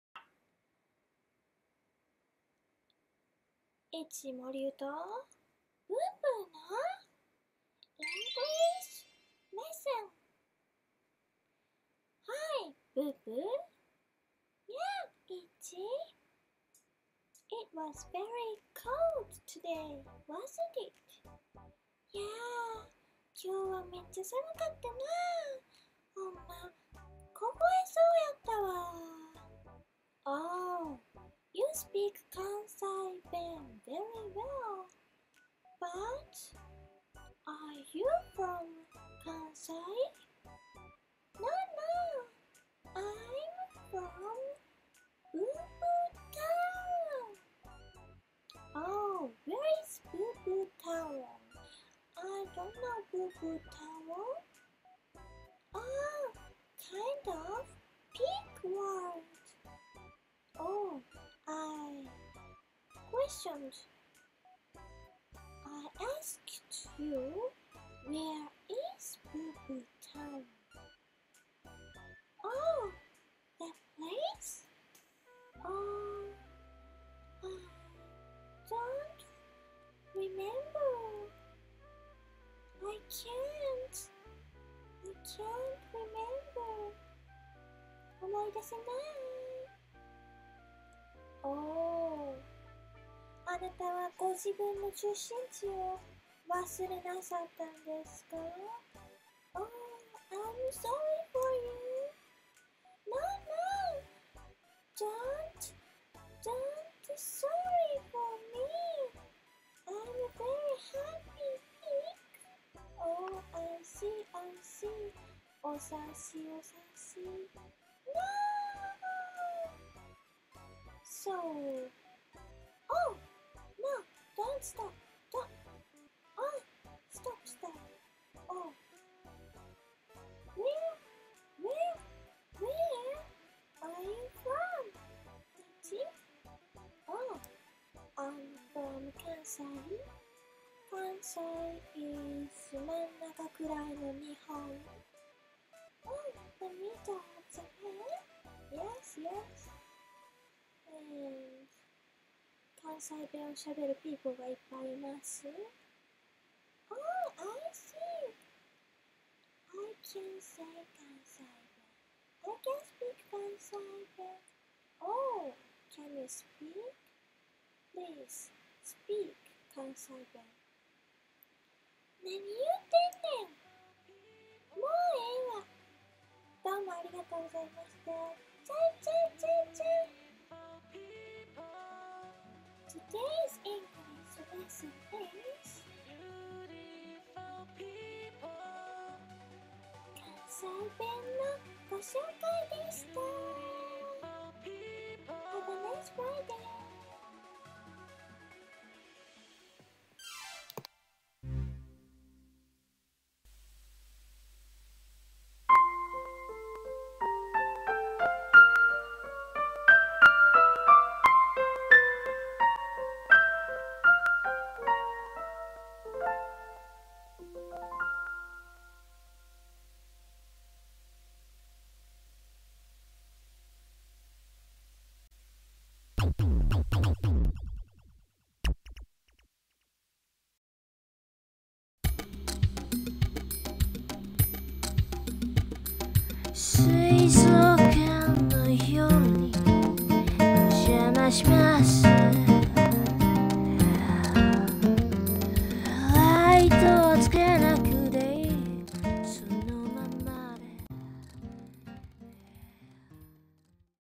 ICHI & PIG 英会話レッスン【関西弁】
ICHI MOLIWと書いてイチモリユと申します。
(My name is ICHI MOLIW.)

【ライブ出演-Live in Japan-】
☆２/１１(月)吉祥寺RJGB
☆２/１４(木)四谷アウトブレイク

雪寸前の東京、今夜もわたしはBoo Booと半熟Englishでトークです。
字幕の関西弁で、もっぱら遊んでいます。

(Please follow me & give your good button.)

チケット取り置き
お仕事依頼↓(Tickets & Works)